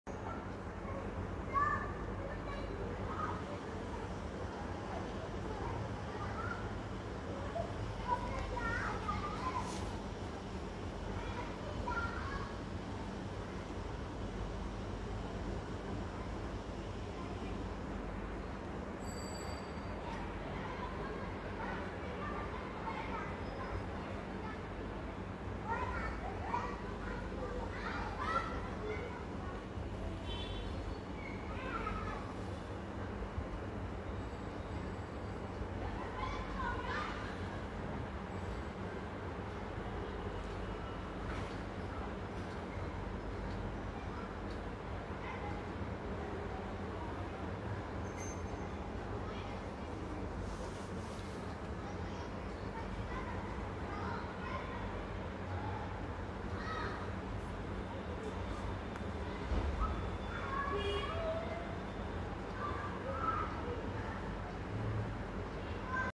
cute cat video#funniest cat video#cat in the box#youtubeshorts#shorts
Louis stuck in the box and now thinking what to do